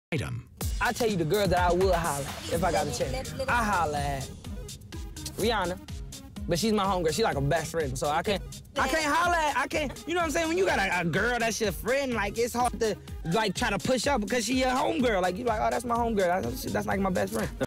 Chris talks about Rihanna
a very very young  Chris talks about Rihanna friendship...